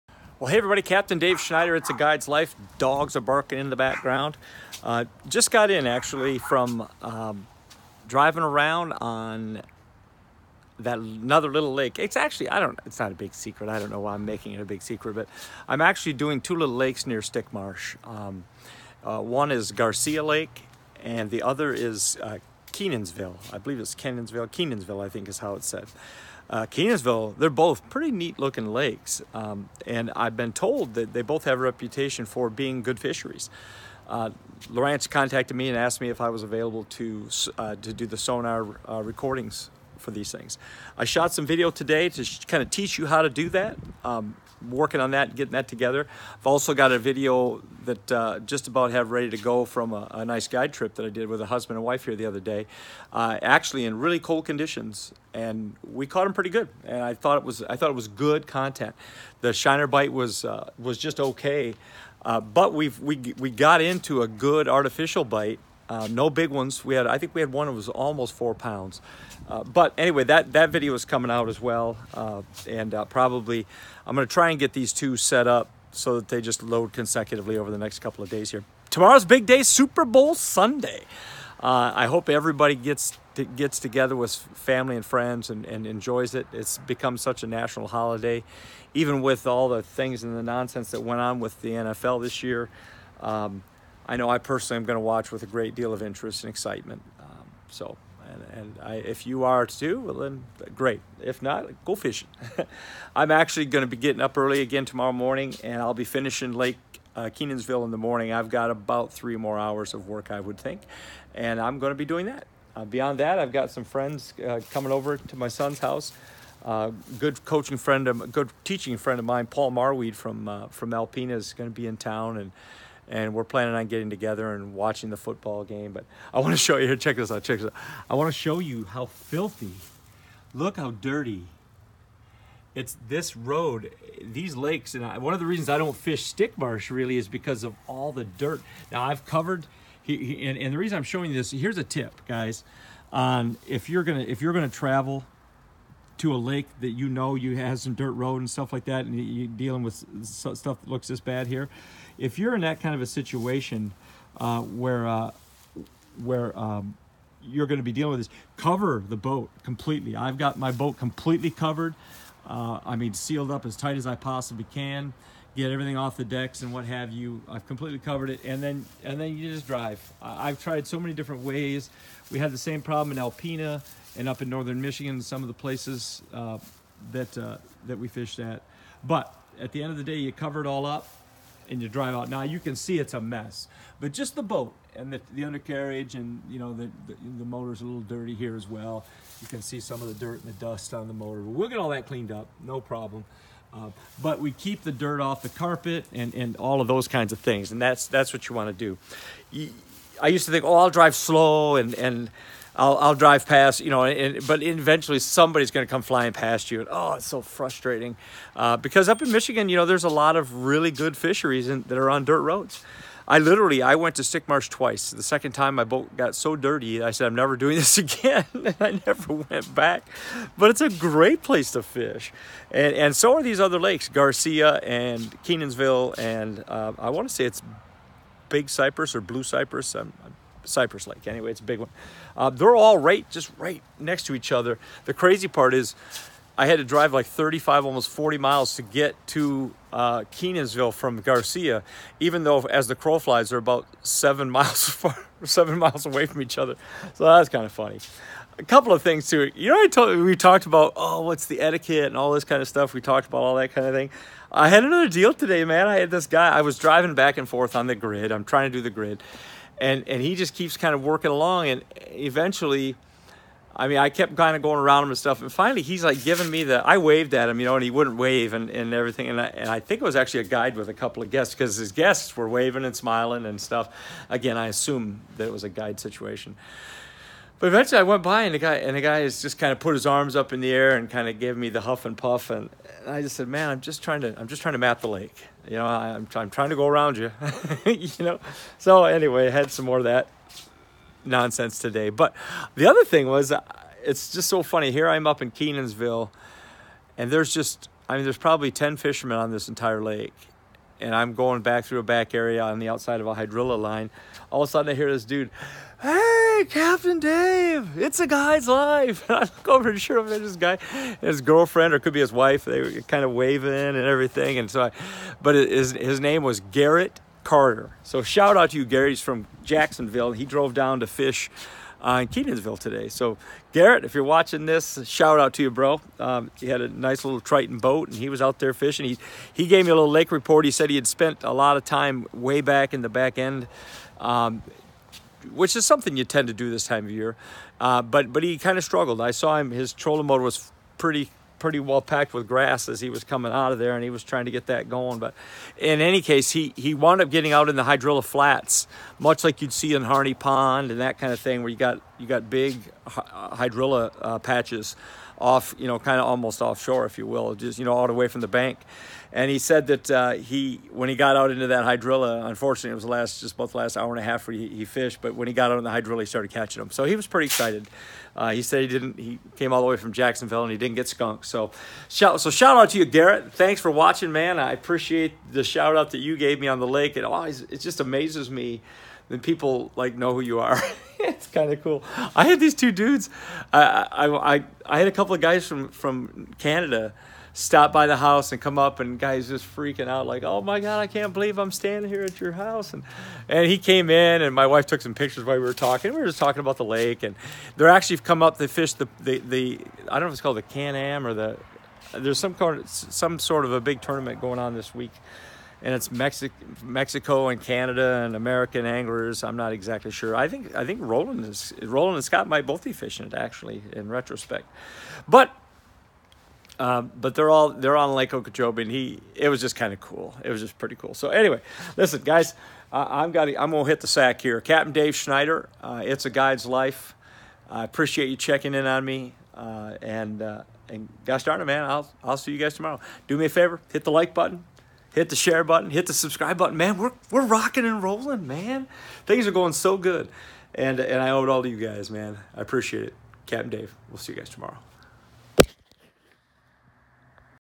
Prepare your boat for dirt road towing
Couple of important tips for keeping your fishing rig in fishing shape when you reach the lake